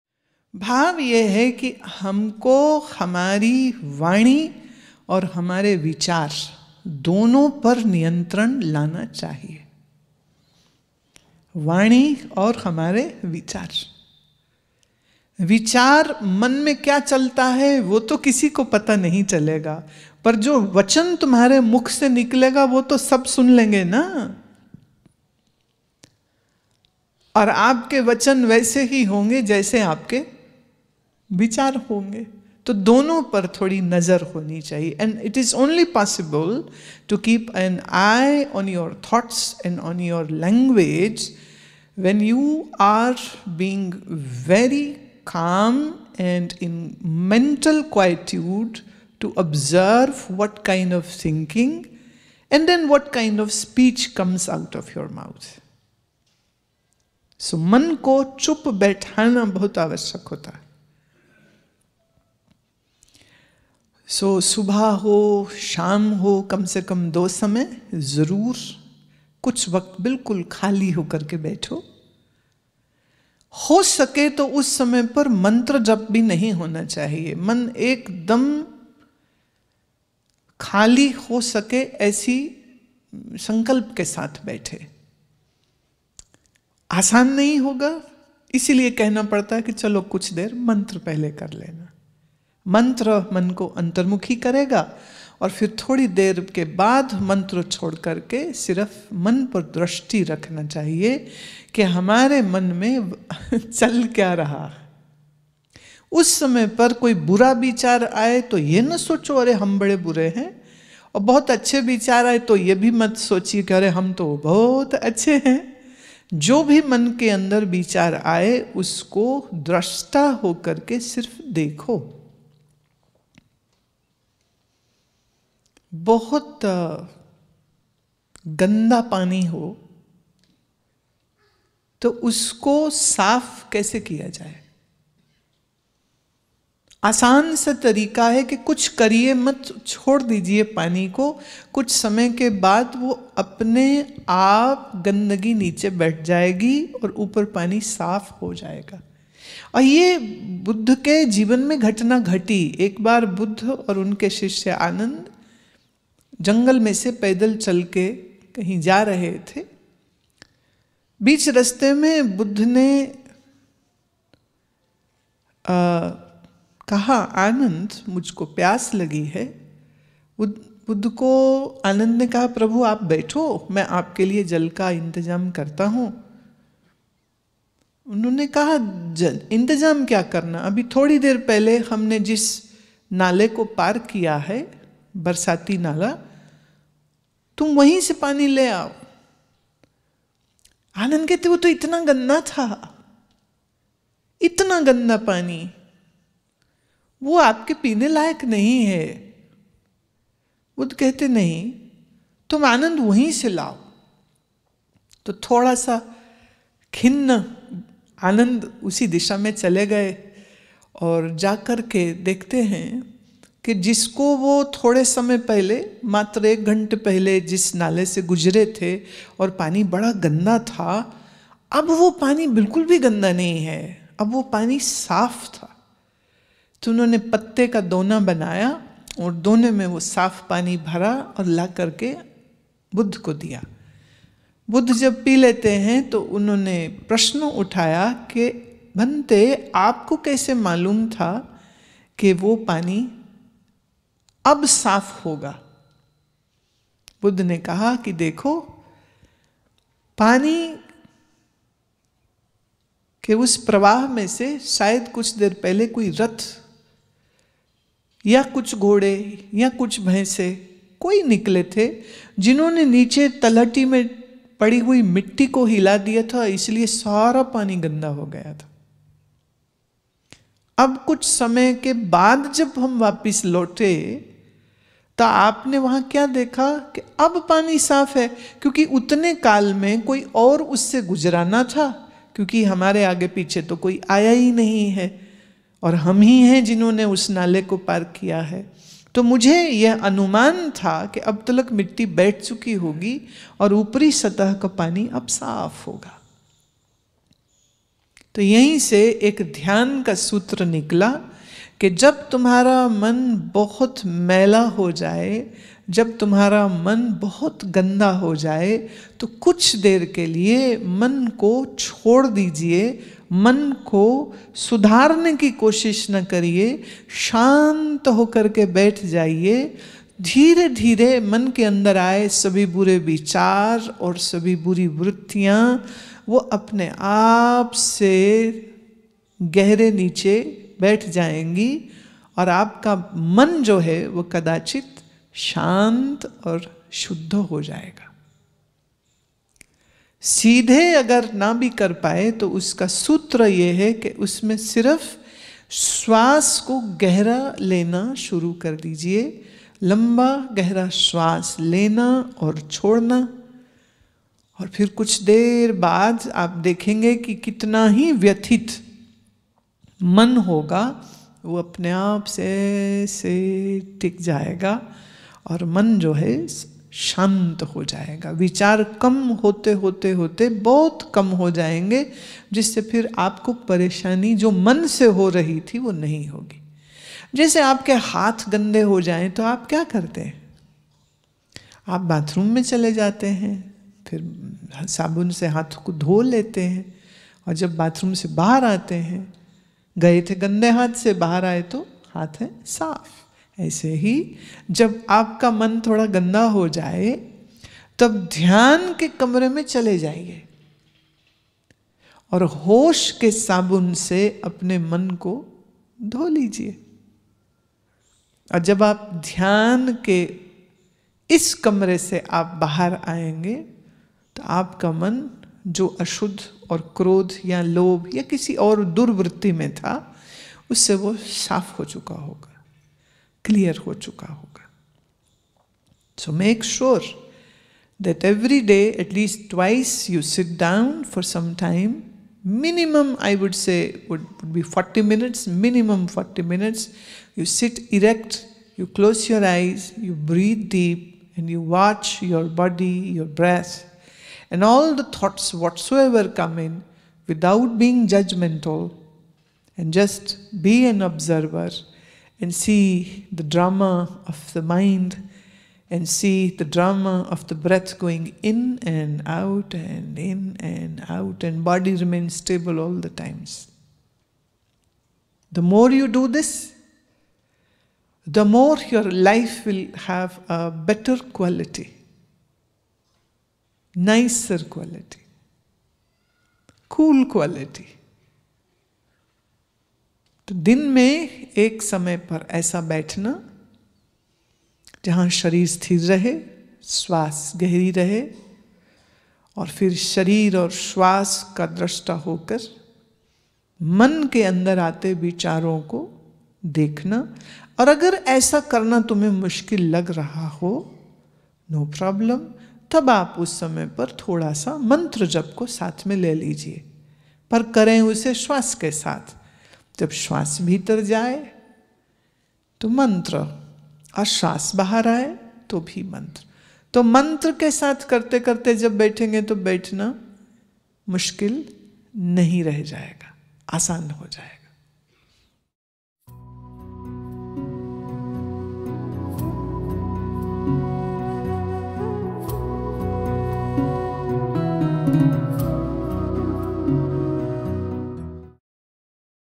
मन को शुद्ध कैसे करें ? How to purify mind? | Anandmurti Gurumaa (with English subtitles)
In our daily interactions with others, usually the mind becomes clouded with overthinking, anger, greed & a plethora of various emotions. Narrating a beautiful story from the life of Buddha & his disciple Ananda, revered master Anandmurti Gurumaa gives priceless words of wisdom & effective ways to purify one's mind.

Wisdom for modern minds through Yoga, meditation, vedanta, ayurveda, astrology and much more…